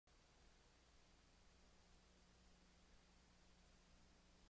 WZZM 13 ABC station ident 1992
Commercial from a VHS recording of Steven King's It miniseries on the 3rd of August, 1992 on WZZM 13 Grand Rapids ABC